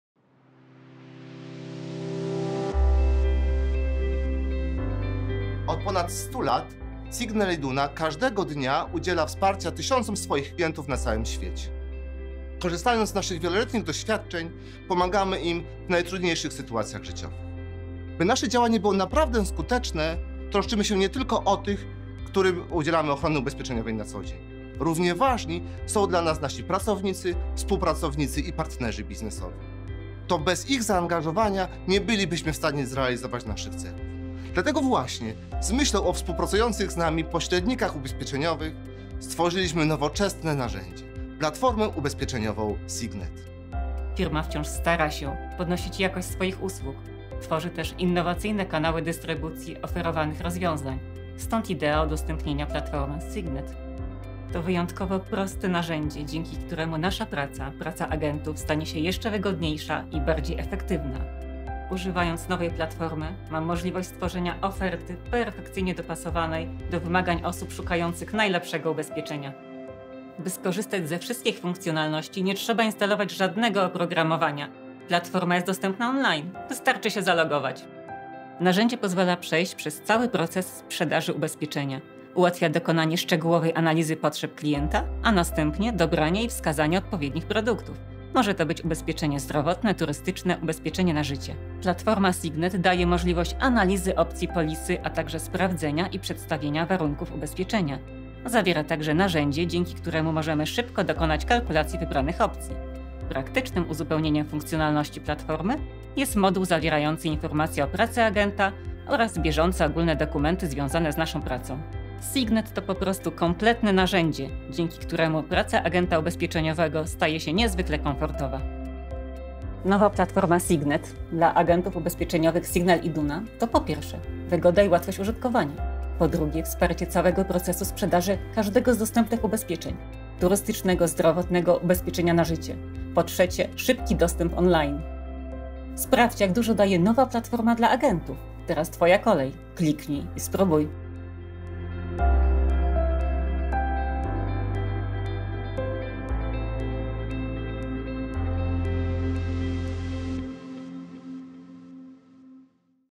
Signal Iduna - SIGnet
Prezentacja możliwości platformy SIGNet dla agentów ubezpieczeniowych Signal Iduna. SIGnet to narzędzie ułatwiające pracę agentów i wspierające proces sprzedaży ubezpieczeń.